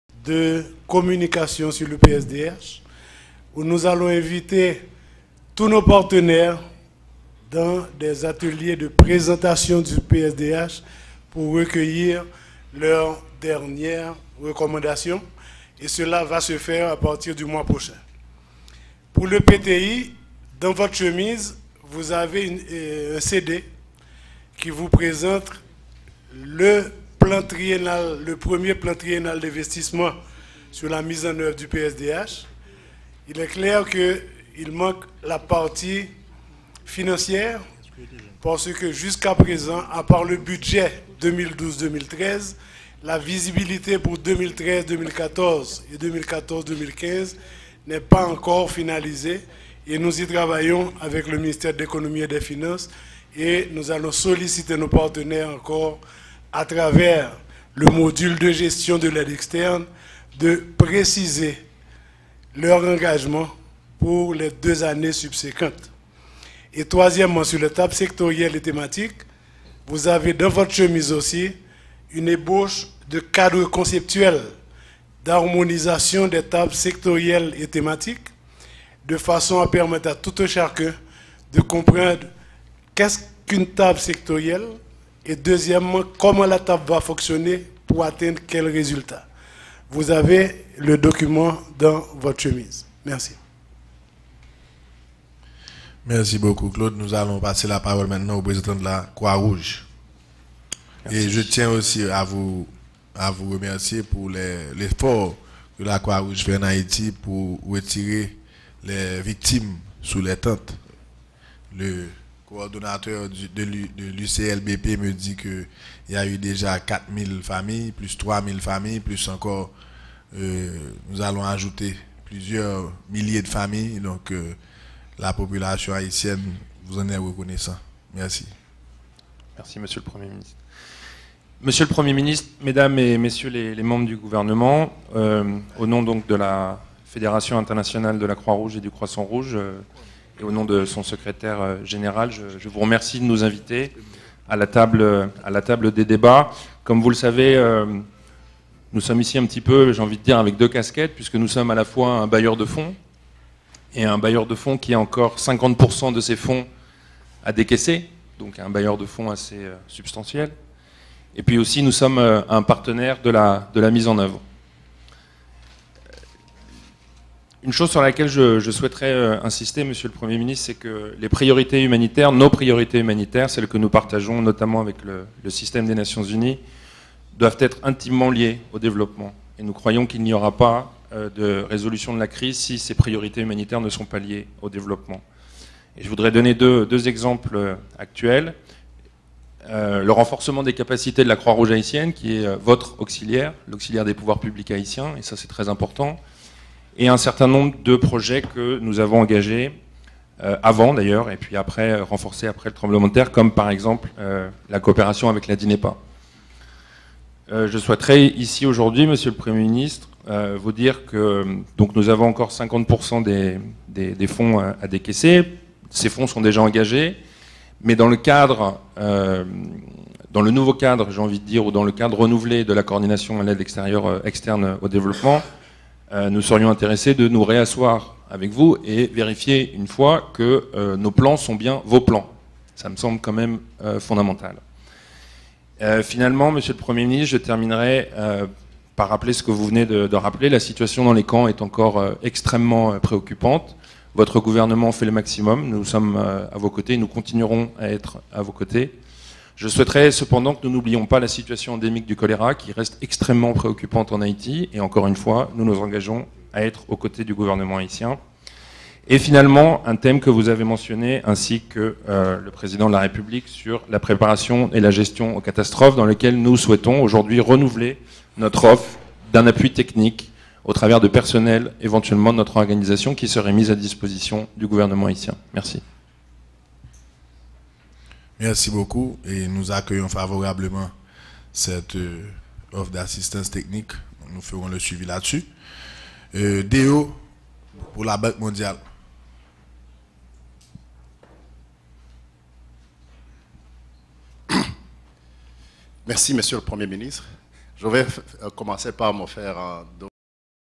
IFRC HoD opening statement CAED Haiti, Alexandre Claudon de Vernisy
International Red Cross opening statement CAED Haiti, Nov 26th Port au Prince